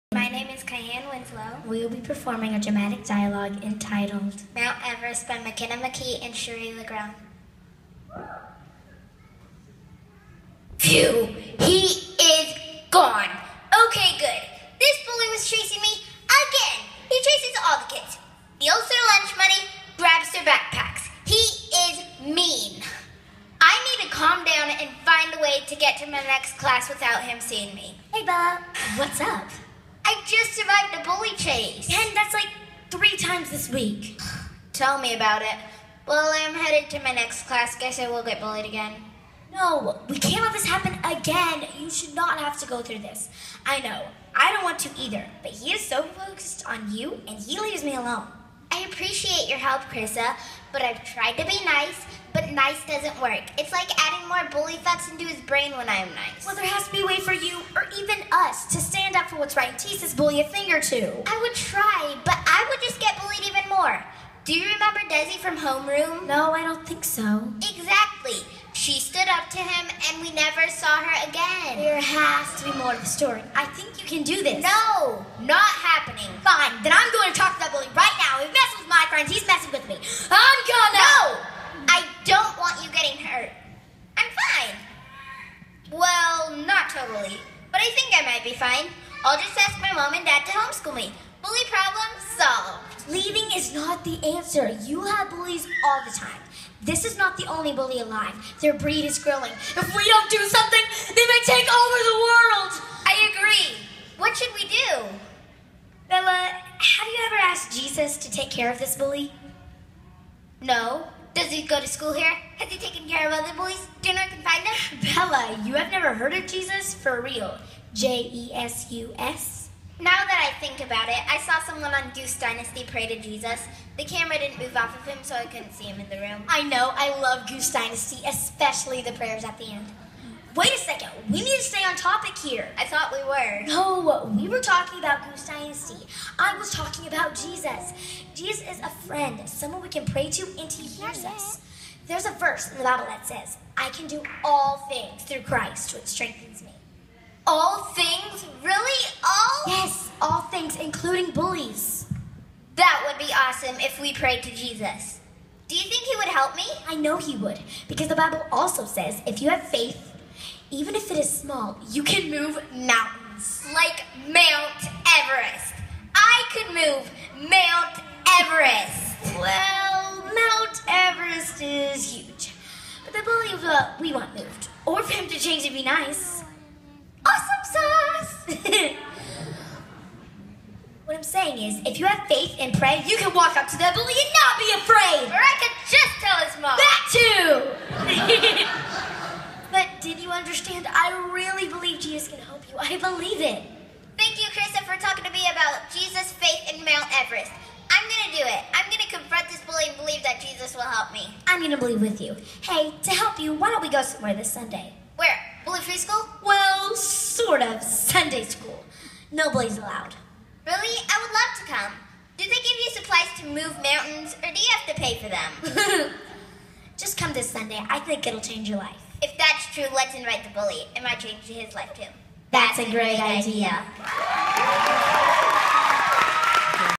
Mount Everest Dramatic Dialogue
2014 KCA Entertainment Night performance by McKenna and Kaianne